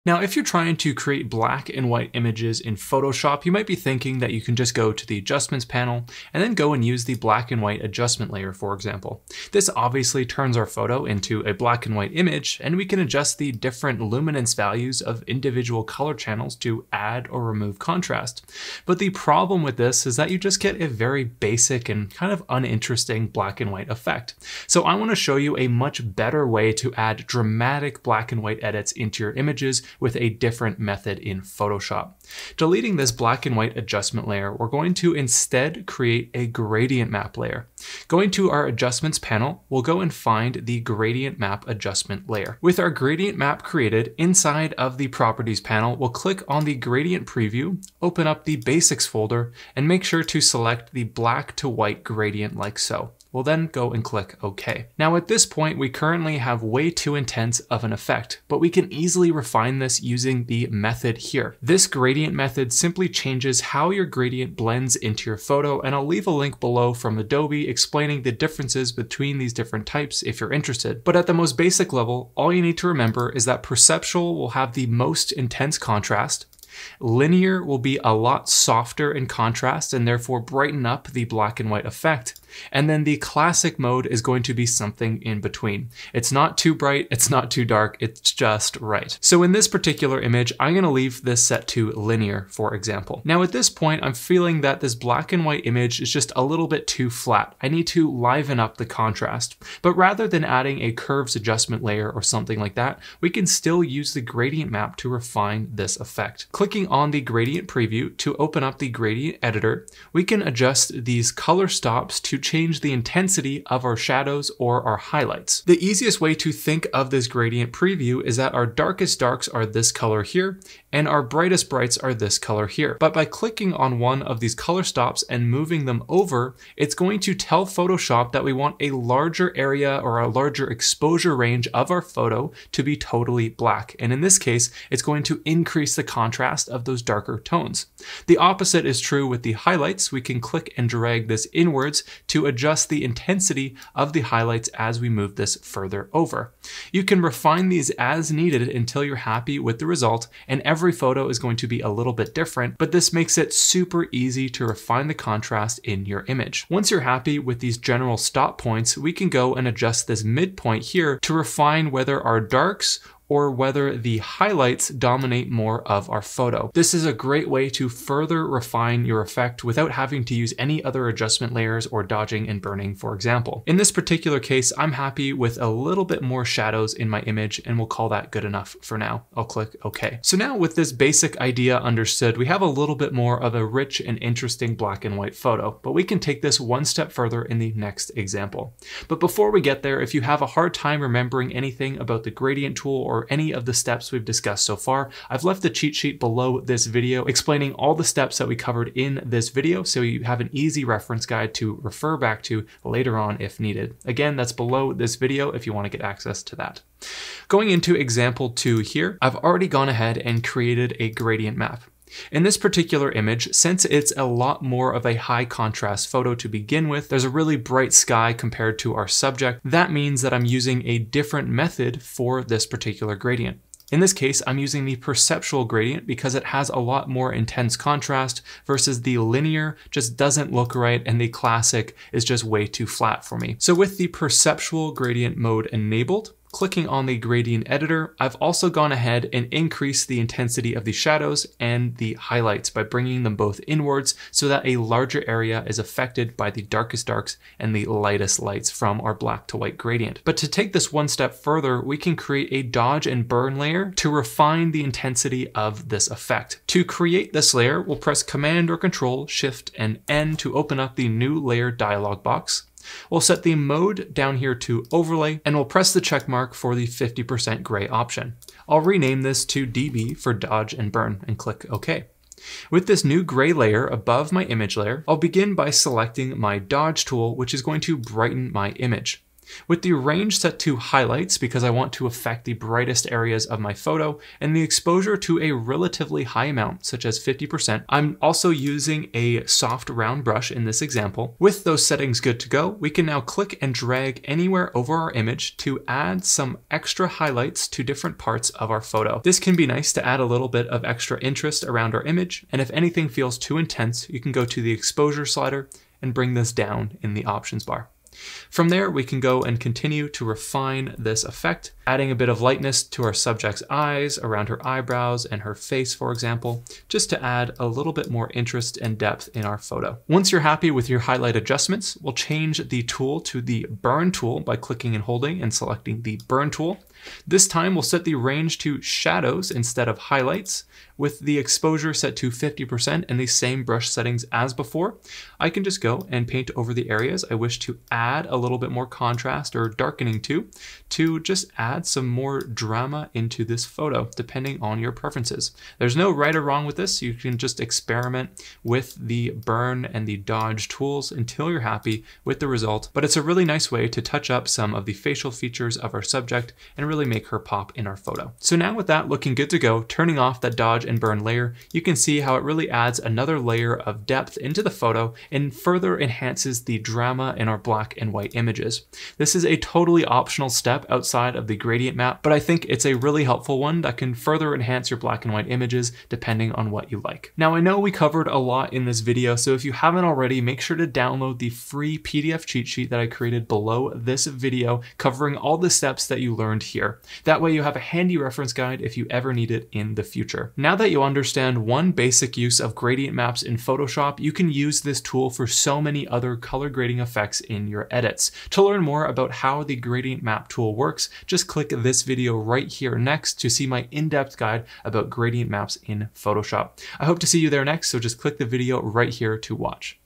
The Secret To DRAMATIC Black & White Images In Photoshop
Tired of flat black and white images? Try this technique in Photoshop!

Timestamps:
0:00 The Problem With Regular BW Edits
0:36 Using Gradient Maps For Better Black & White Edits
1:38 Adding Contrast With Gradient Maps
3:14 Adding Extra Drama To Black And White Edits
Learn how to edit black and white photos in Photoshop using a slightly different method than you might expect. Rather than using the Black and White adjustment layer, this lesson shows you the advantage of using Gradient Maps instead to create dramatic black and white images that are more captivating and eye-catching. With the addition of dodging and burning, these techniques are the ultimate way to make stand-out black and white effects in any image! Enjoy :)